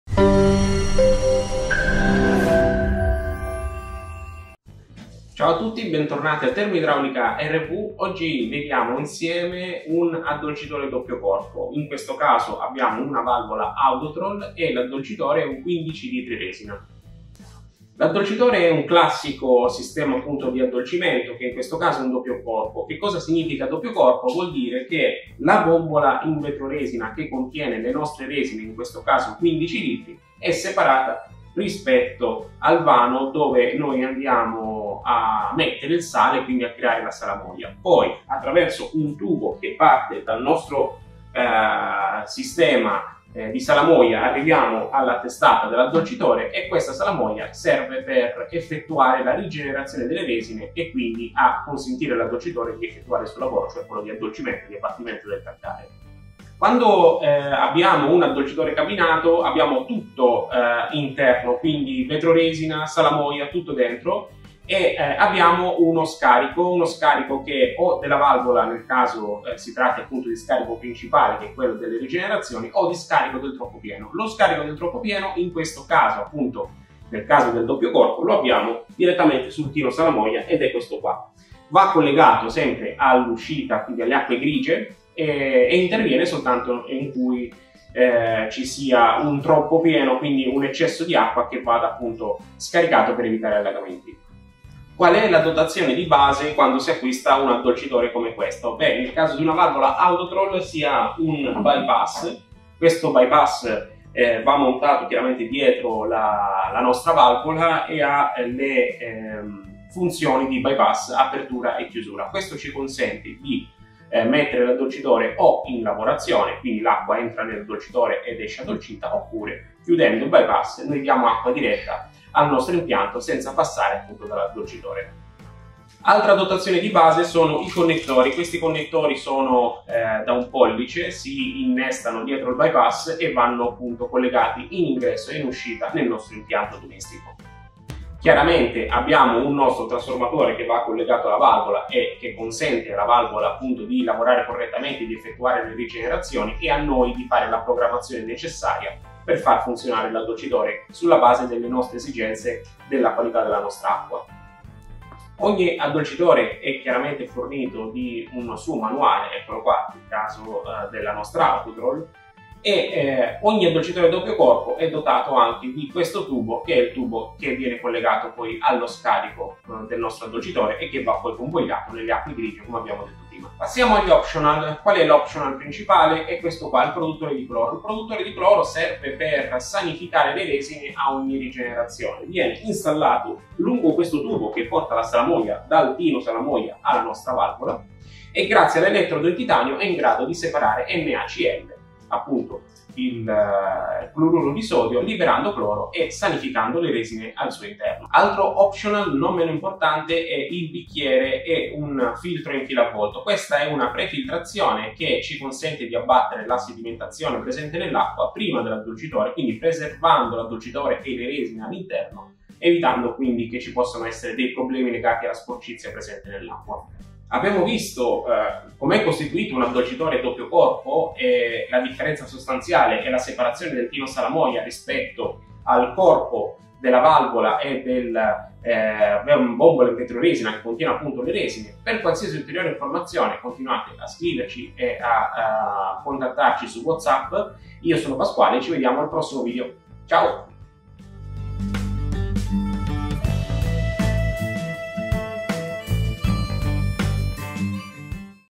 Addolcitore doppio corpo con valvola Autotrol - Praticità ed efficienza
Gli addolcitori doppio corpo sono sistemi ideali per impianti da appartamento o anche condominio e danno la possibilità di regolare la durezza in uscita.
Installato a monte di un impianto, grazie alle ampie portate, sono in grado di erogare acqua addolcita.
Gli addolcitori sono compresi di By-pass e di alimentatore 12/230V – 50Hz spina europea.

*Benefici e vantaggi*
Riduzione della durezza dell’acqua, che è causa di incrostazioni di calcare sugli elettrodomestici quali lavastoviglie, lavatrici, lavandini e molto altro.

*Risparmio economico*
Riduzione dell’uso di saponi, ammorbidenti e prodotti chimici. Costi di manutenzione ridotti. Funzionamento completamente automatico. Meno uso di casse d'acqua e quindi riduzione di bottiglie di plastica.
15lt. resina. (adatto per 3/4 persone)
– Capacità di interscambio: 90ºHFxm3.
– Consumo sale a rigenerazione: 2,5 Kg./sale.
– Consumo acqua a rigenerazione: 100,5lt.
– Portata max. 0,68m3/h
– Dimensioni ØxA (mm.) 196X1.087
– Bombola – 15lit.: 7″x35″
– Tino sale ØxA (mm.) 340×905 – contenuto sale 75lit.

00:00 Intro 🎬
00:04 Addolcitore doppio corpo con valvola Autotrol da 15 litri 💧
00:55 Scarico nel Tino Salamoia ⚙️💧
01:34 Valvola Autotrol by-pass ⚙️
02:06 Connettori e kit installazione per l'addolcitore doppio corpo ⚙️
02:53 Produttore di Cloro 💧
03:18 Bicchiere e filtro in filo avvolto ⚙️
03:37 Contatti 📬